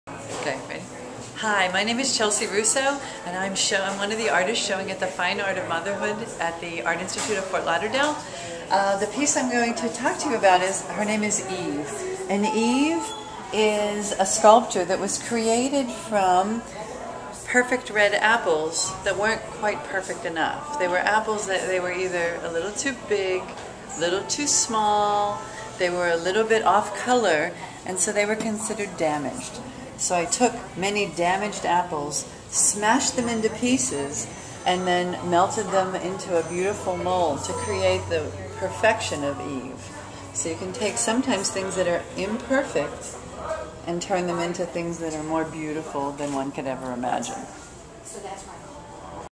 Fine Art of Motherhood - Chelsea Rousso and Eve.MOV
Chelsea Rousso talks about "EVE" in the latest exhibition in the Mark K. wheeler Gallery at The Art Institute of Fort Lauderdale running through Sept. 1, 2010.